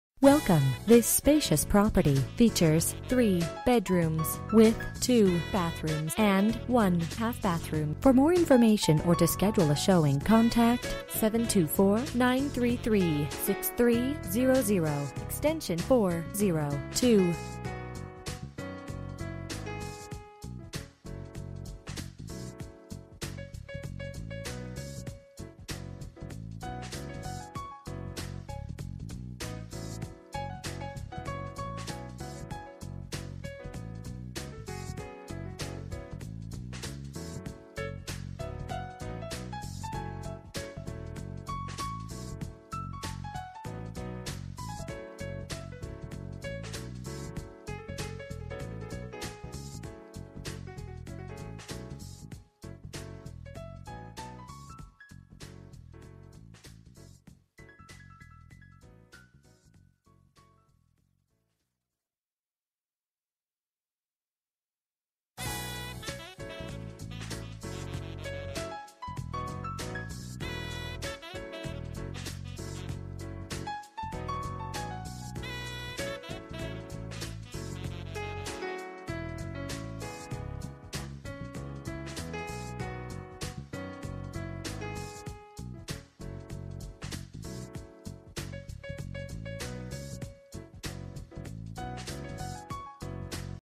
Residential for sale - 349 Oak Road, Plum Boro, 15239
Well maintained split level home!  Updated Gameroom with new drywall, new recessed lighting, freshly painted & new carpet!  Updated Powder Room!  Brand new 100amp electric service panel.  14X13 master bedroom with full bath & new carpet! Nice size kitchen with all appliances included.  Access to rear patio from kitchen.  Nice size laundry/utility room with washer and dryer included! New Garage Doors with Keypad! Nicely landscaped yard with fenced rear yard!  One-year HSA Home Warranty with 7 Star Buyer Upgrade included!

Bedrooms: 3
Bathrooms: 2 full and 1 half Bathrooms
Square Feet: 0
Price: $163,900

For more information about this property, please contact Cindy Palmer at x.402 or . You can also text 3938238 to 67299.

10/12/2016 03:07:01 pm

Last modified: